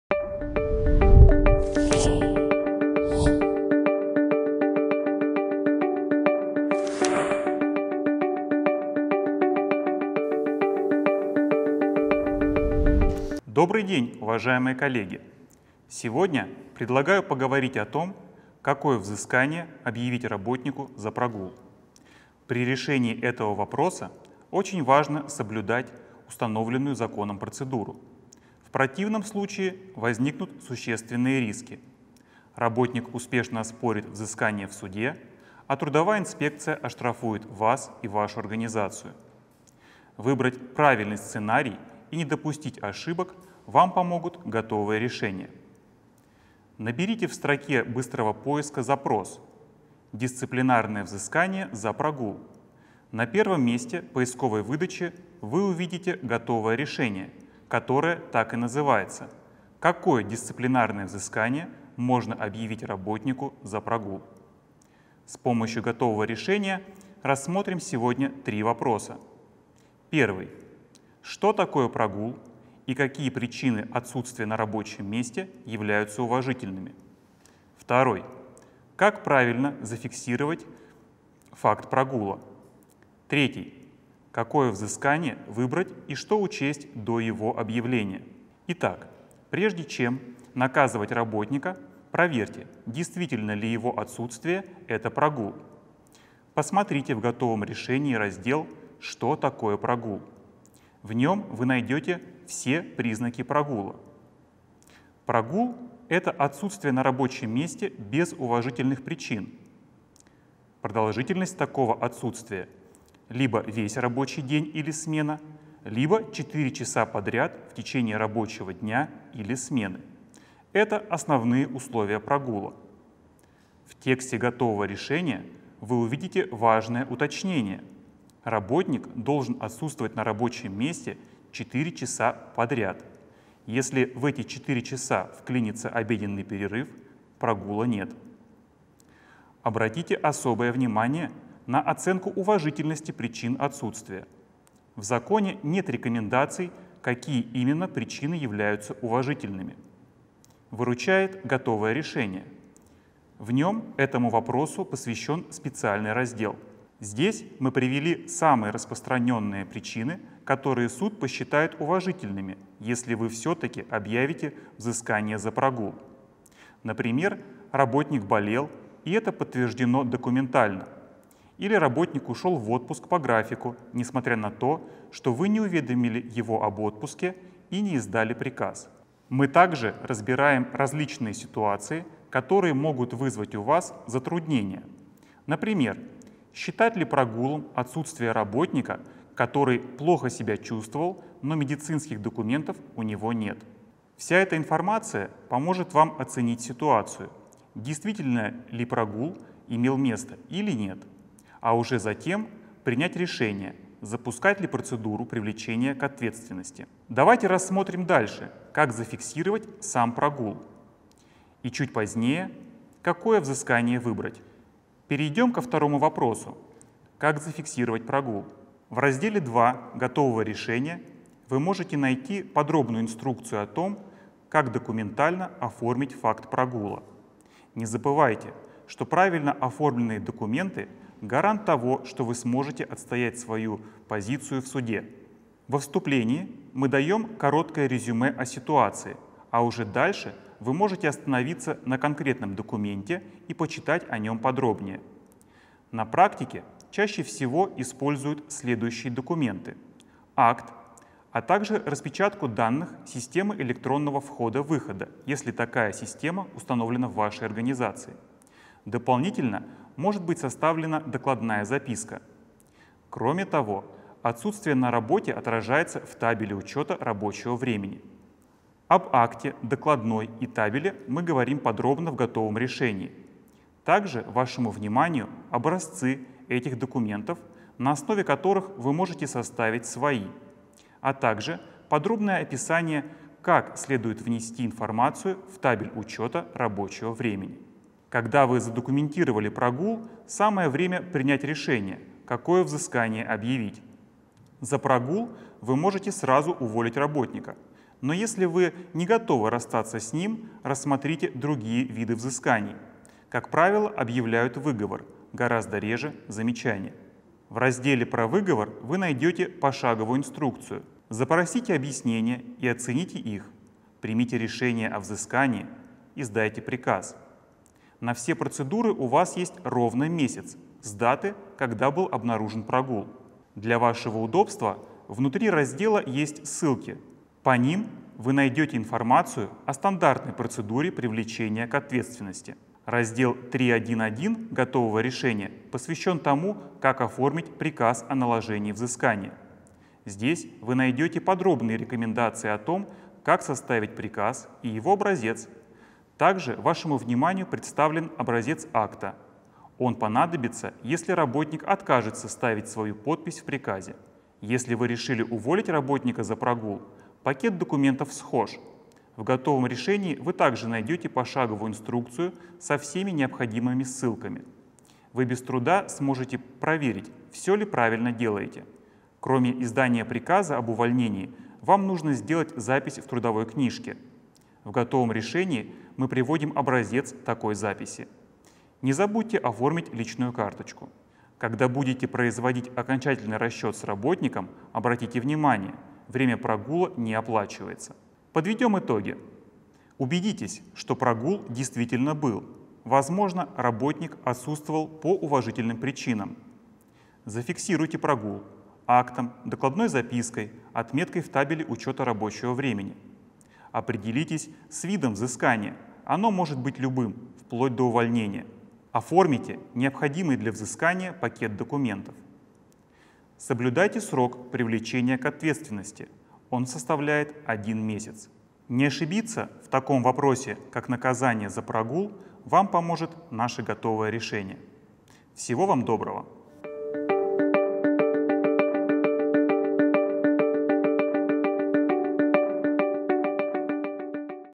Какие наказания предусмотрены за прогул: ответы в Готовых решениях
Действительно ли отсутствие на работе подпадает под определение прогула? Как зафиксировать факт прогула? Какое дисциплинарное взыскание выбрать? Лектор подскажет, как с помощью Готовых решений найти ответы на эти и другие вопросы.
Лектор – Гаранжа Станислав Александрович, эксперт в сфере трудового права, юрист-аналитик КонсультантПлюс, кандидат юридических наук.